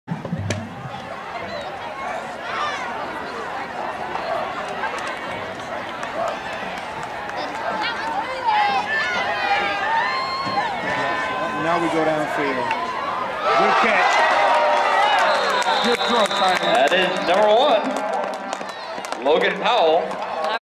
Northrop Bruins 08
#4 Colin Bouillon Fires a 27 yard completion to #1 Logan Powell in a game between The Northrop Bruins & the Northside Redskins